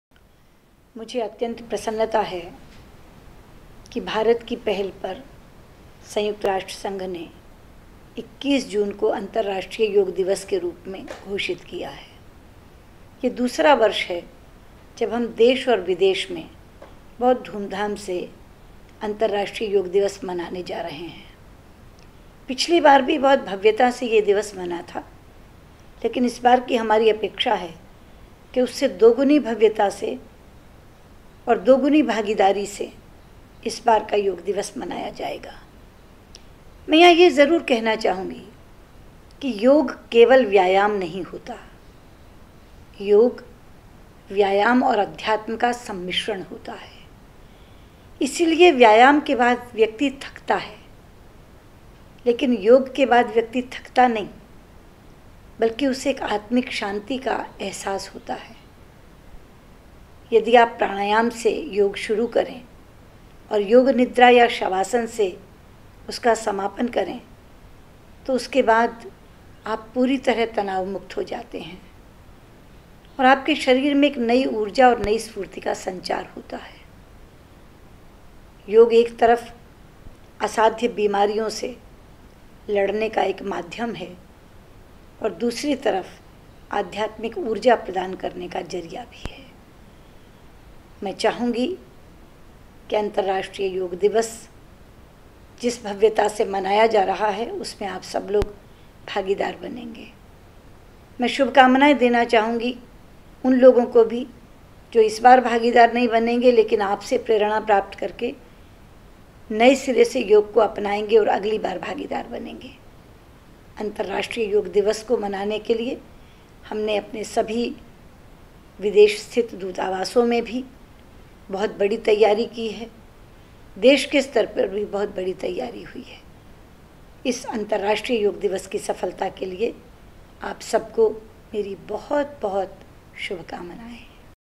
(Portuguese subtitled) Message by External Affairs Minister Sushma Swaraj on 2nd IDY
Message on 2nd International Day of Yoga by External Affairs Minister Sushma Swaraj (with Portuguese subtitles)